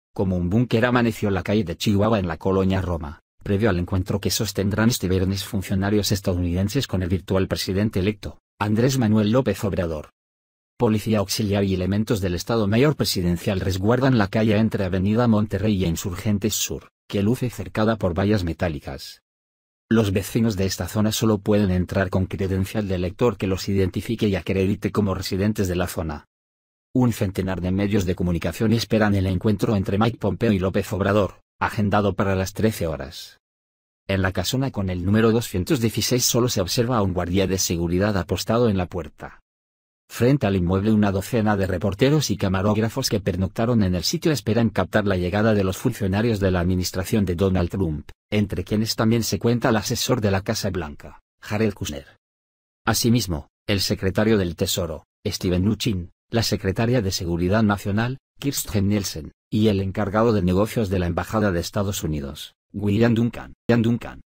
Oficina de AMLO es un bunker por visita de Mike Pompeo y Yerno de Trump
Como un búnker amaneció la calle de Chihuahua en la colonia Roma, previo al encuentro que sostendrán este viernes funcionarios estadounidenses con el virtual presidente electo, Andrés Manuel López Obrador.

Policía auxiliar y elementos del Estado Mayor Presidencial resguardan la calle entre Avenida Monterrey e Insurgentes Sur, que luce cercada por vallas metálicas.

Los vecinos de esta zona sólo pueden entrar con credencial de elector que los identifique y acredite como residentes de la zona.

Un centenar de medios de comunicación esperan el encuentro entre Mike Pompeo y López Obrador, agendado para las 13:00 horas.

En la casona con el número 216 sólo se observa a un guardia de seguridad apostado en la puerta.
Frente al inmueble una docena de reporteros y camarógrafos que pernoctaron en el sitio esperan captar la llegada de los funcionarios de la administración de Donald Trump, entre quienes también se cuenta al asesor de la Casa Blanca, Jared Kushner.

Asimismo, el secretario del Tesoro, Steven Mnuchin: la secretaria de Seguridad Nacional, Kirstjen Nielsen; y el encargado de negocios de la embajada de Estados Unidos, William Duncan